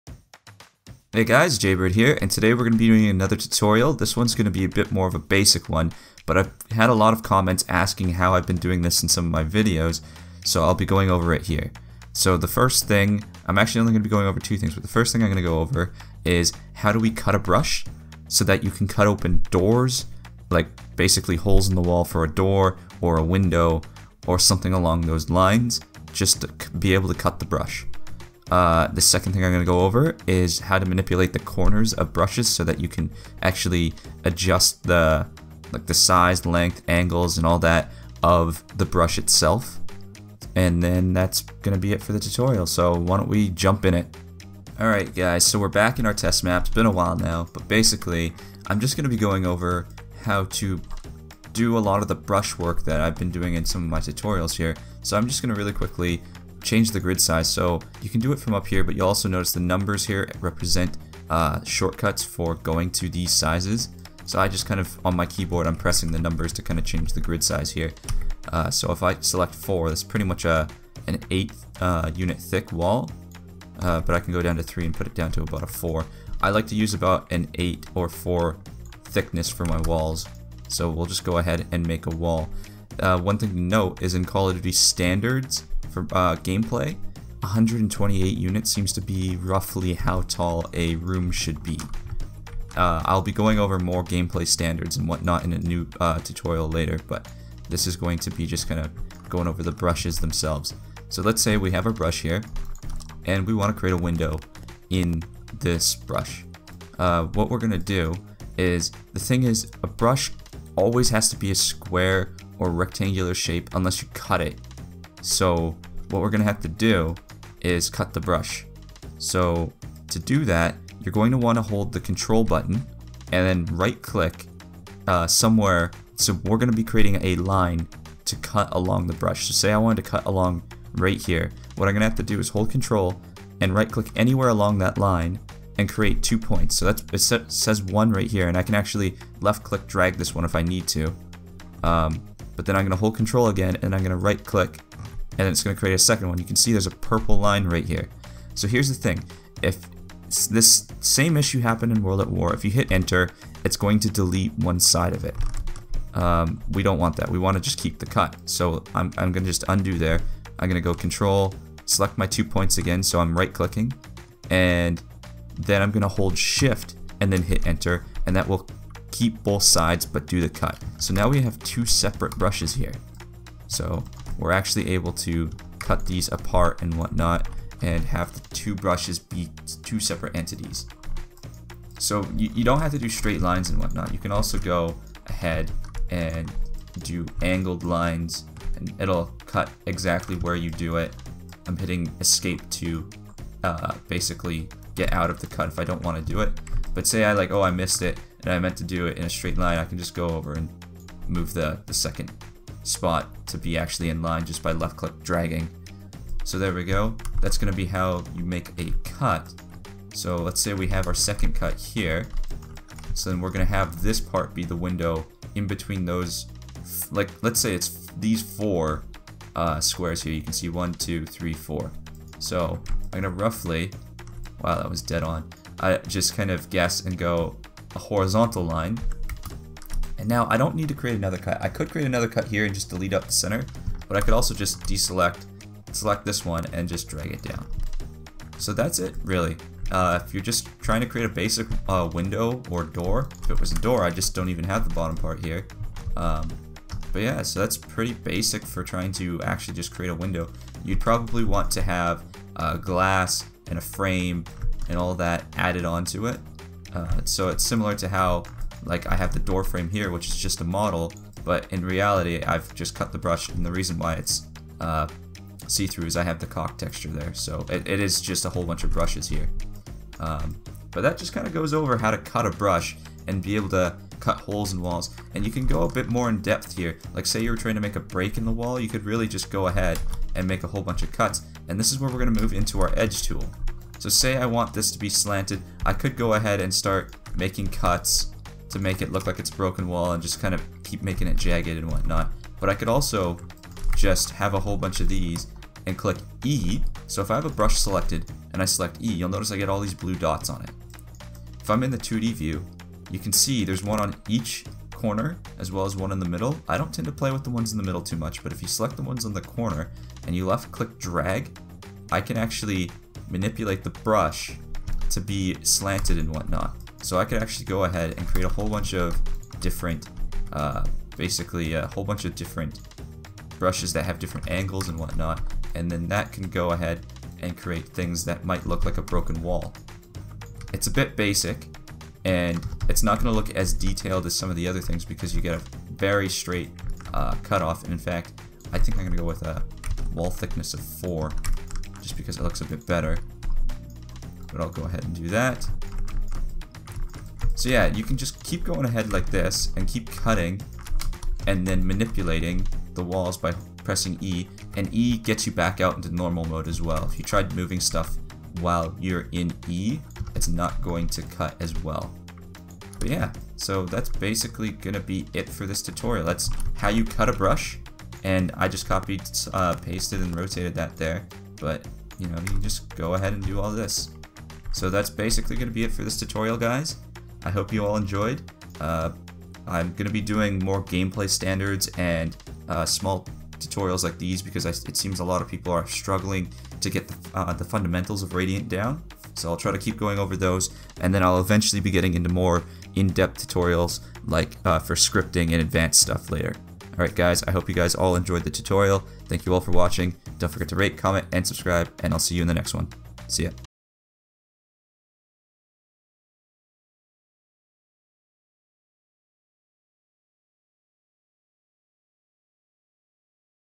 Black Ops 3 Mod Tools | Tutorial Part 18 - Brush Editing
This is more of a basic tutorial, but I seem to be getting a lot of questions in the comments section about this - so I decided to make a quick tutorial on brush editing.